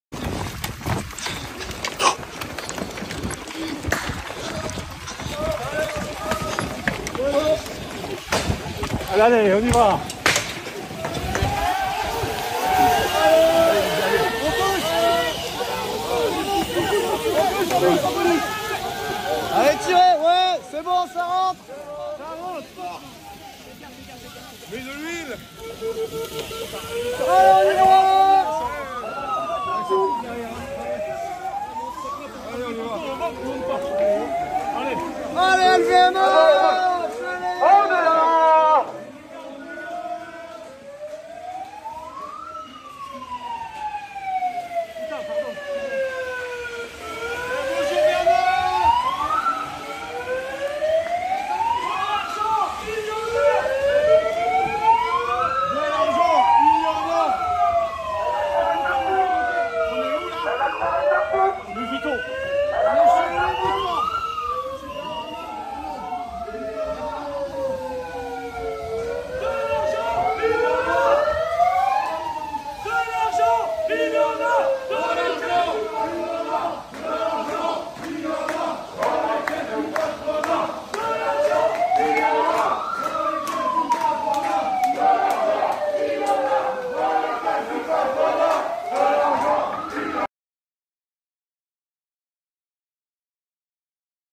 French pension protesters invade Louis Vuitton headquarters in Paris
As protests against pension reforms continued in France on April 13, a group invaded the offices of the luxury goods conglomerate LVMH in central Paris.

Footage by Mike Ruiz shows protesters rushing into the building, and letting off flares as alarms go off.

LVMH founder Bernard Arnault was named this year as the richest person in the world by Forbes.